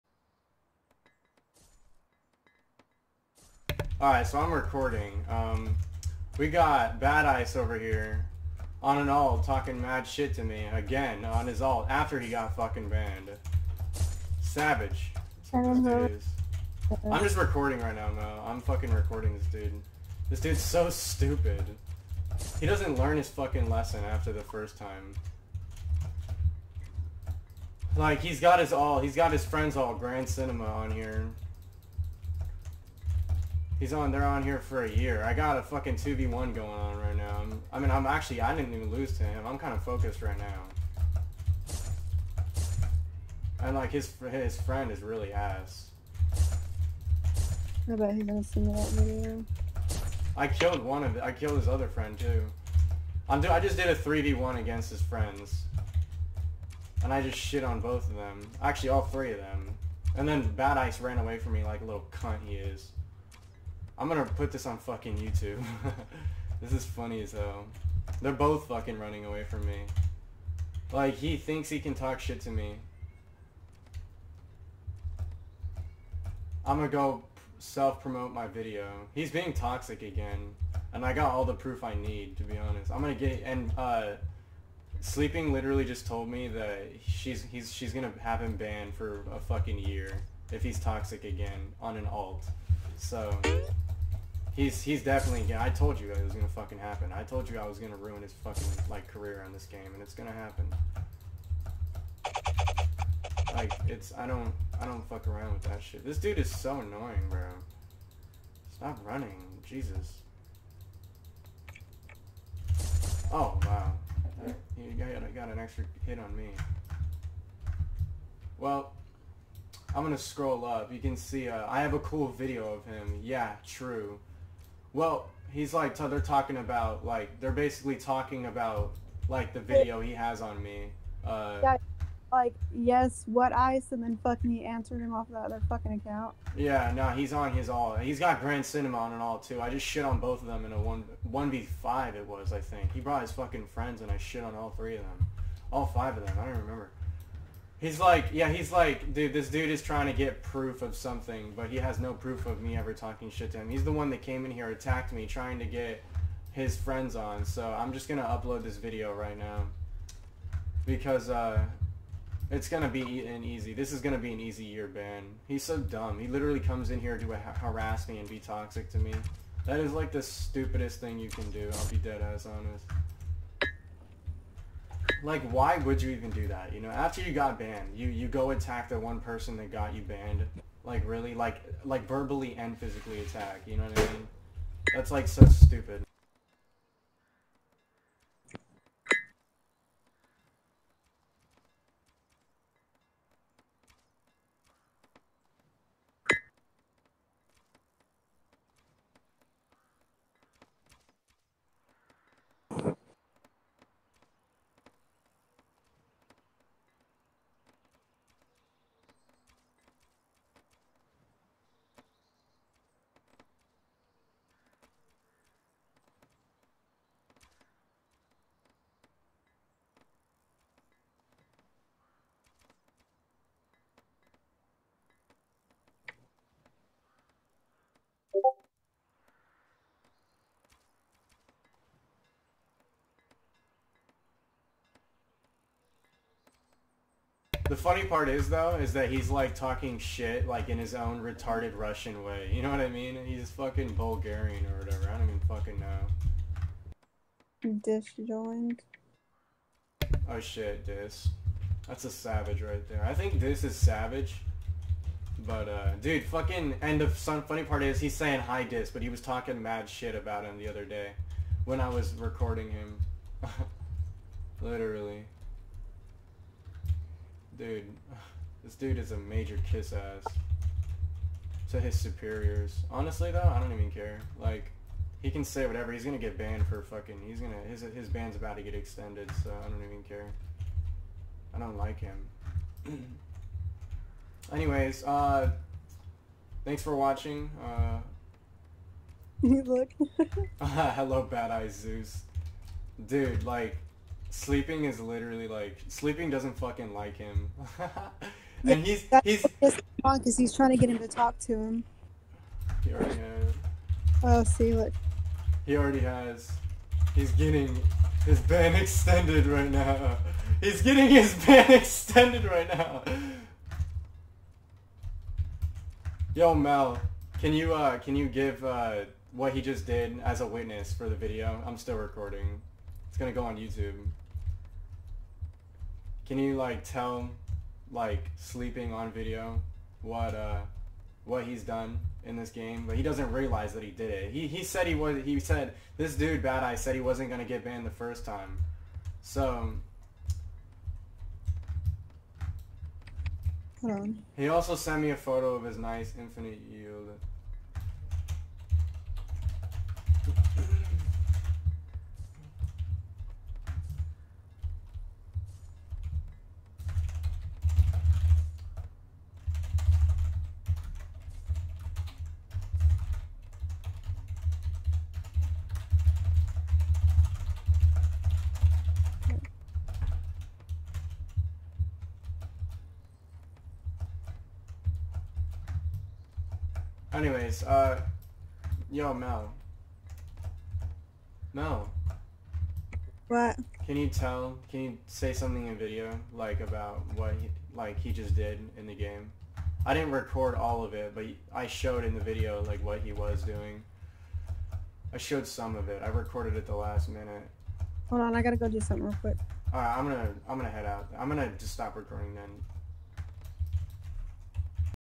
badice and his friends poopooed on/ badice toxicity on alt v2
well. here we go again. alts vs me apparently.

he's got his friends on an alt, he's on an alt. he gets shit on, they get shit on. he runs, they fight; they lose. he talks mad shit in the glade, he leaves.

badice i thought you'd have learned your lesson by now but apparently you didn't so let this be your last reminder.